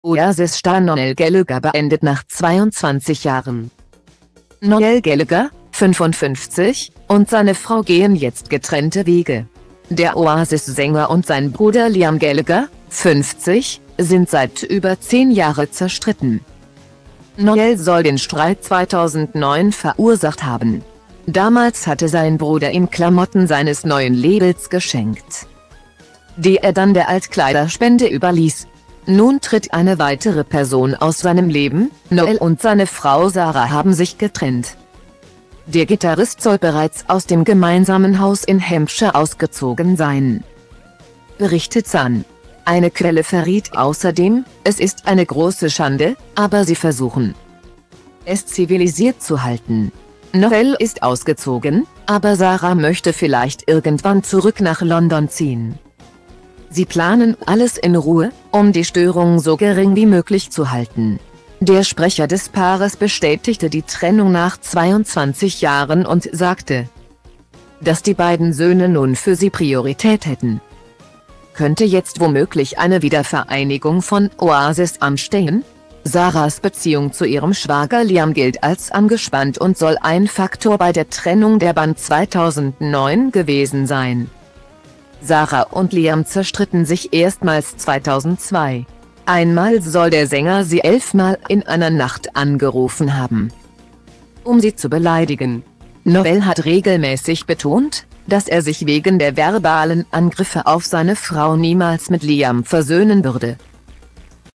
Oasis-Star Noel Gallagher beendet nach 22 Jahren
Noel Gallagher (55) und seine Frau gehen jetzt getrennte Wege. Der Oasis-Sänger und sein Bruder Liam Gallagher (50) sind seit über zehn Jahre zerstritten.Noel soll den Streit 2009 verursacht haben. Damals hatte sein Bruder ihm Klamotten seines neuen Labels geschenkt,die er dann der Altkleiderspende überließ. Nun tritt eine weitere Person aus seinem Leben: Noel und seine Frau Sara haben sich getrennt.